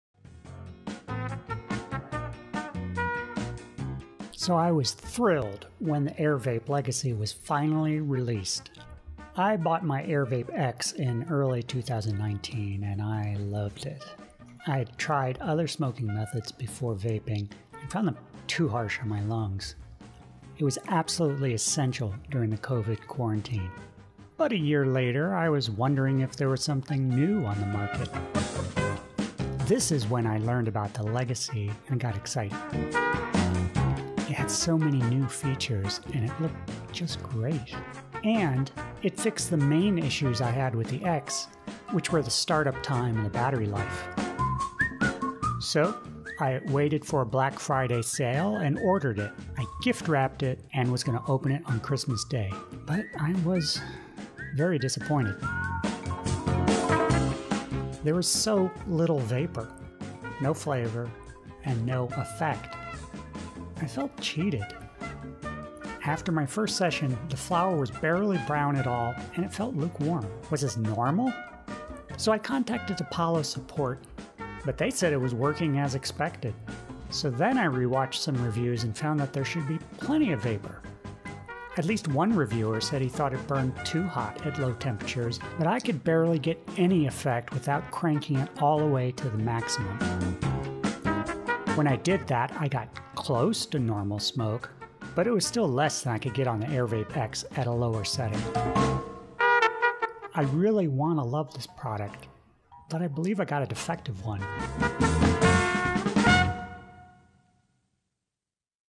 AirVape Legacy Blues
I want to love my new AirVape Legacy, but it produces almost no vapor. Please help!